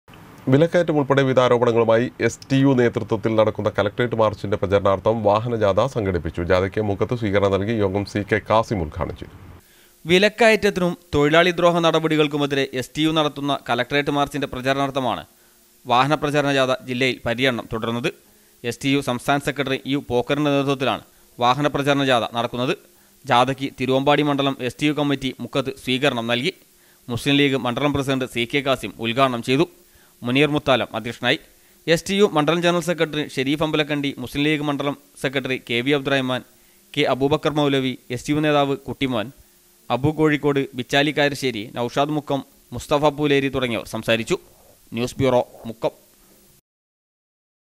എസ് ടി യു നേതൃത്വത്തിൽ നടക്കുന്ന കലക്ട്രേറ്റ് മാർച്ചിൻ്റെ പ്രചരണാർത്ഥം വാഹന ജാഥ സംഘടിപ്പിച്ചു
വിലക്കയറ്റമുൾപ്പെടെ വിവിധ ആരോപണങ്ങളുമായി എസ് ടി യു നേതൃത്വത്തിൽ നടക്കുന്ന കലക്ട്രേറ്റ് മാർച്ചിൻ്റെ പ്രചരണാർത്ഥം വാഹന ജാഥ സംഘടിപ്പിച്ചു. ജാഥയ്ക്ക് മുക്കത്ത് സ്വീകരണം നൽകി. യോഗം സി.കെ കാസിം ഉദ്ഘാടനം ചെയ്തു